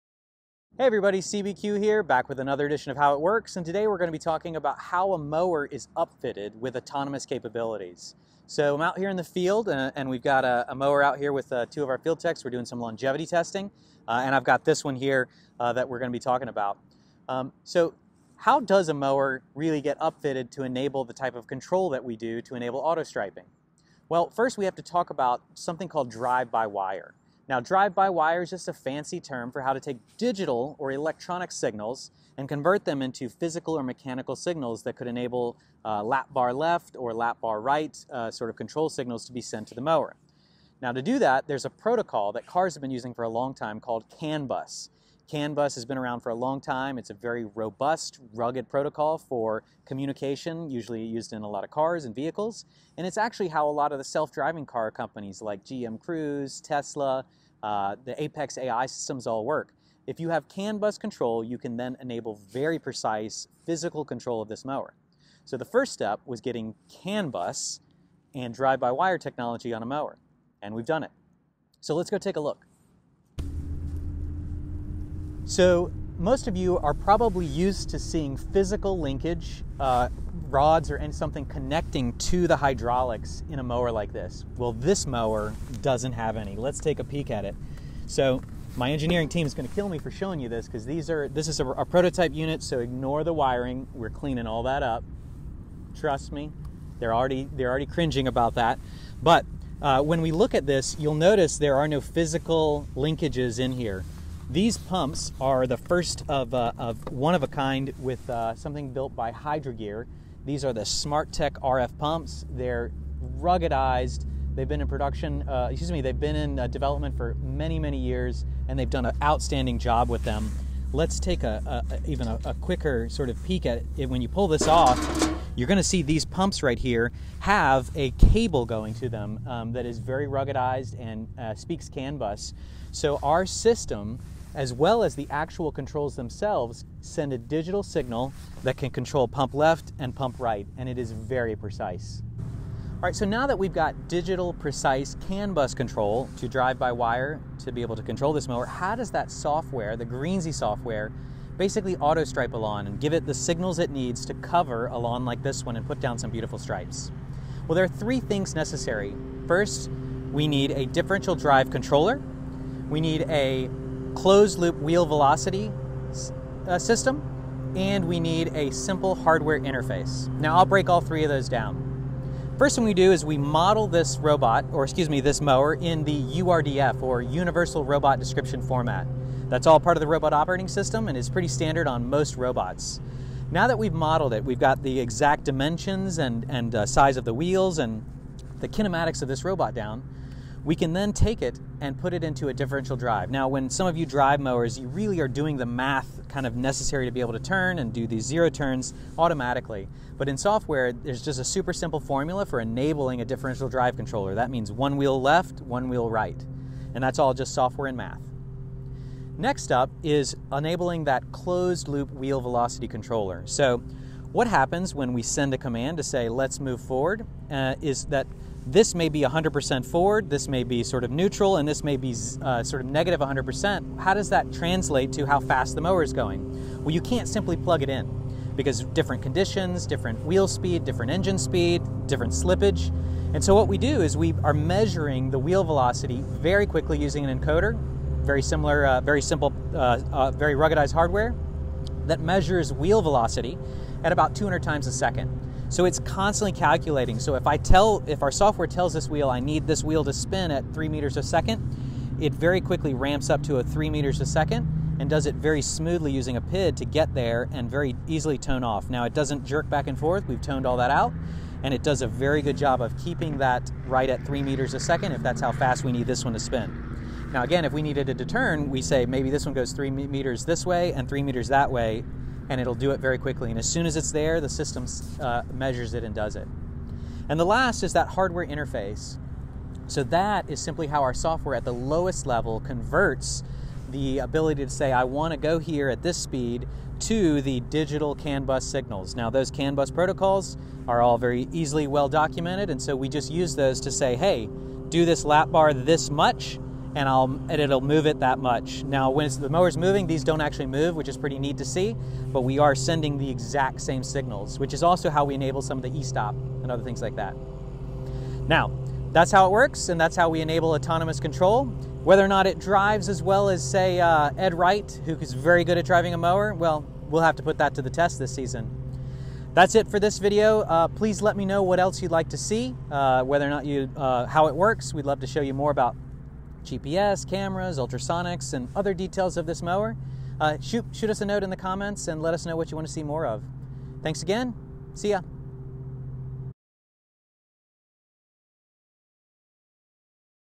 Greenzie Product - How it Works: Autonomous Upfitting
How it works: How does this software make the mower autonomous? What's the process?

We've designed Greenzie to make mowers autonomous and enable 'autostriping'. This video explains how we do that and gives and gives a complete inside look at our mowers software.

Greenzie's official Launch Day is Thursday, Feb 25th at noon.